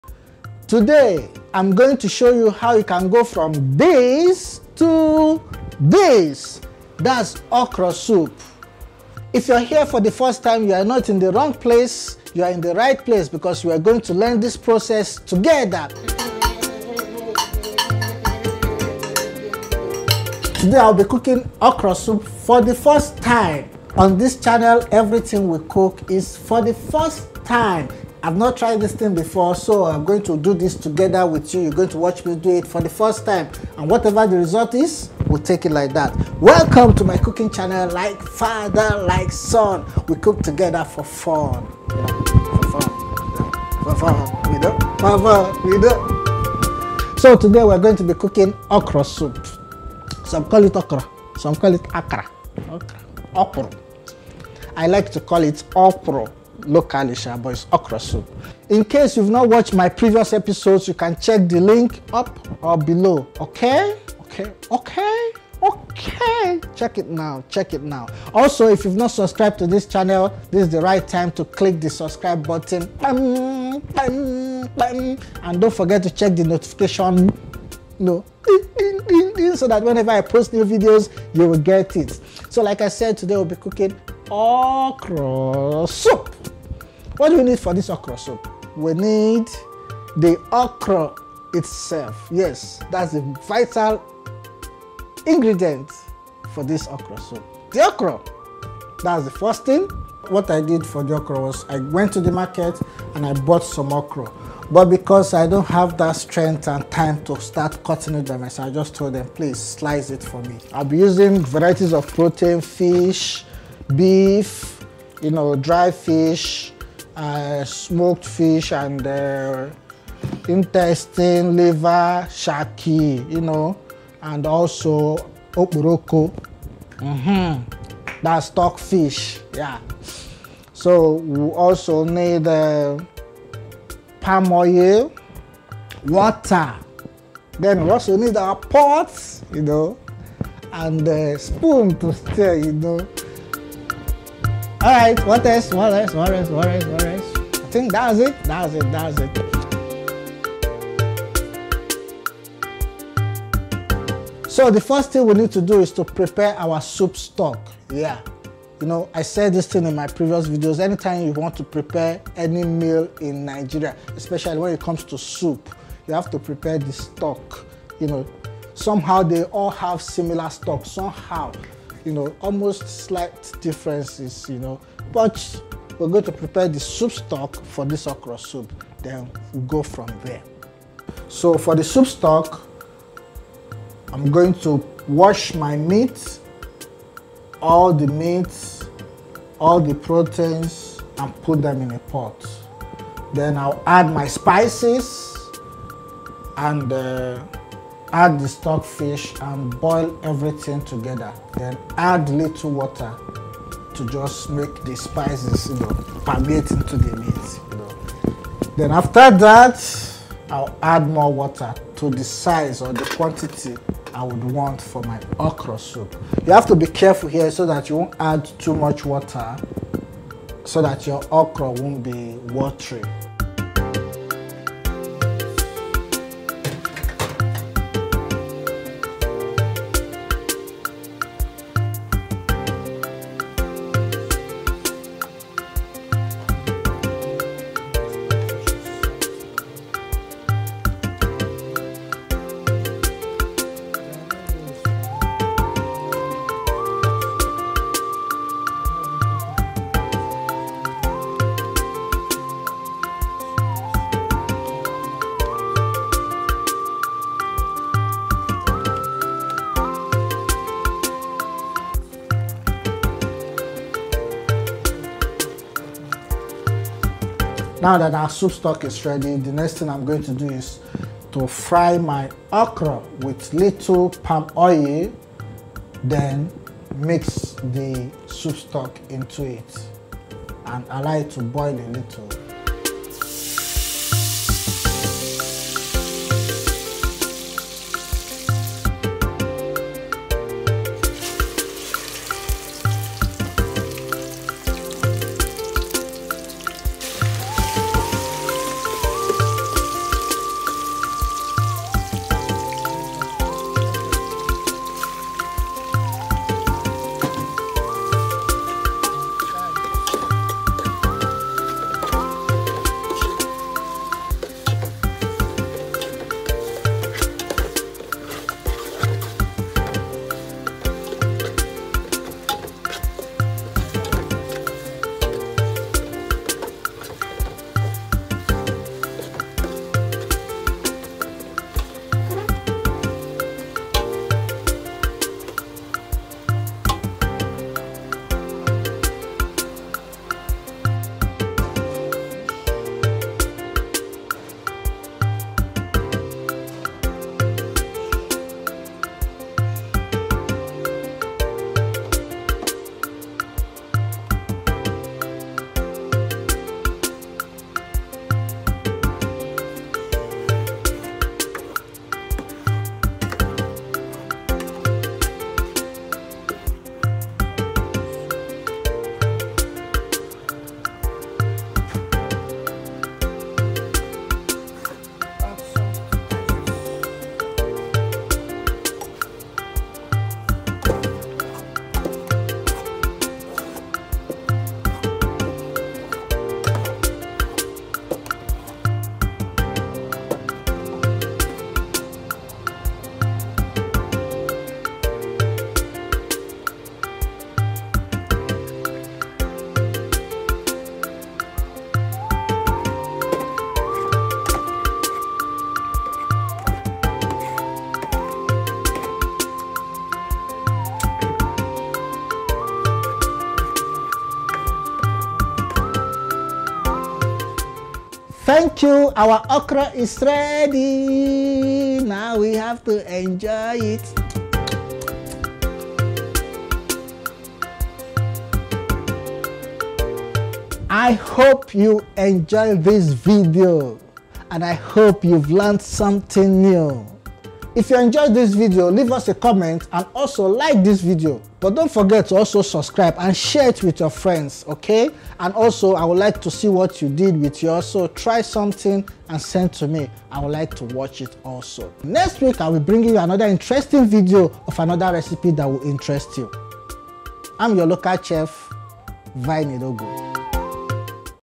Okra Soup Recipe: How To Cook Okra With Palm Oil - A Popular African food can cure any disease
In this video, you will see the step-by-step process and procedure so you can learn how to make your own Okro soup on YouTube without any stress or yawa. If you are a man, I have also made it easy to follow and fun to watch. The ingredients are very simple and readily available.

Okro, Ugu leaf, Palm oil, Beef, Panmo/Kanda, Smoked Fish, Stockfish, Dried fish, Pepper, Salt, Water, Onions and All-purpose spices.

Can a man cook? The cooking is done by a male and not a female. So for me, this cooking is gold.

Filmed with: Canon 5D M4 & iPhone 8
Edited with: Apple Final Cut pro